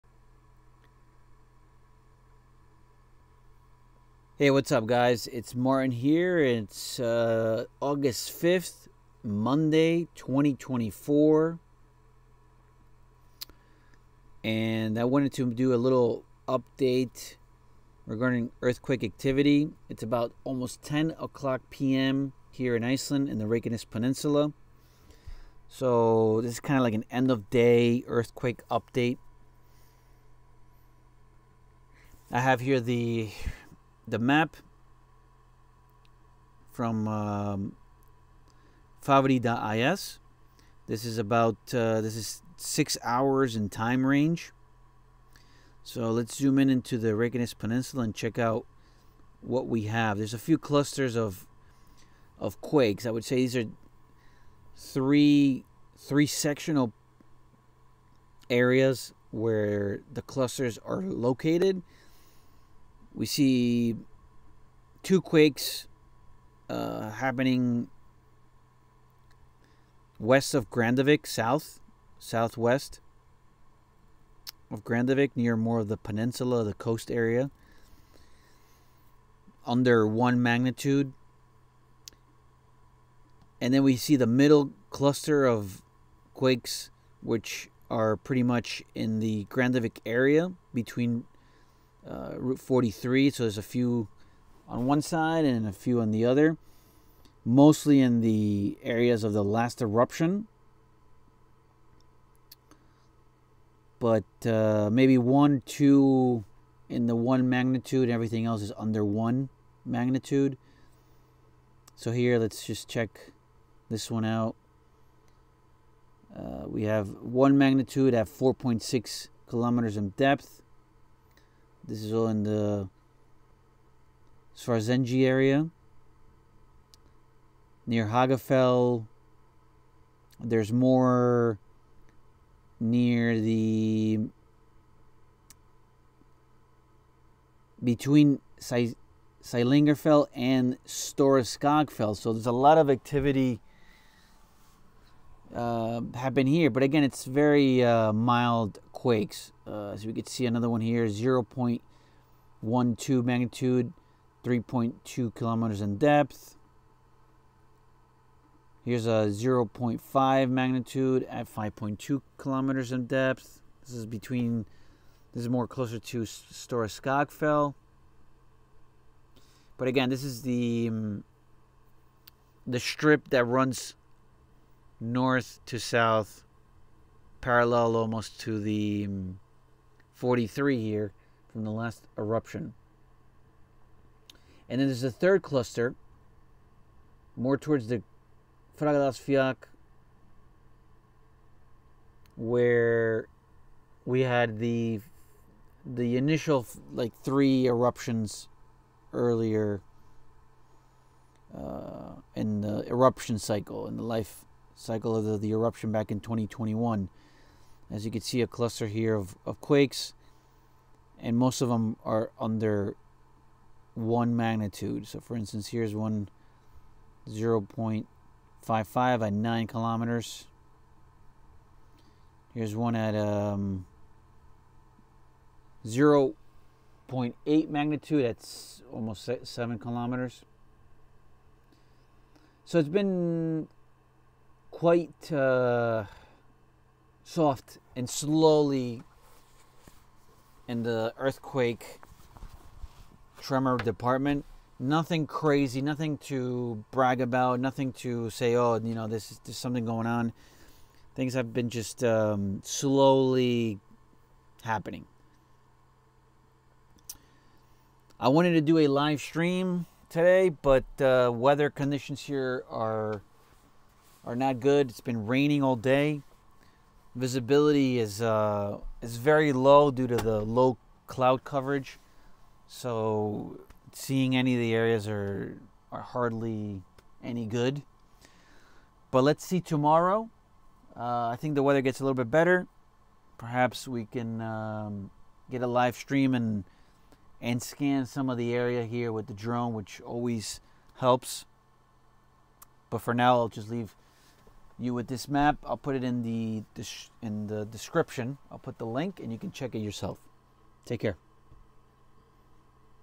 Earthquake check (5 august) - Iceland Eruption 2024
Earthquake activity as of August 5, 2024 around 10pm at night.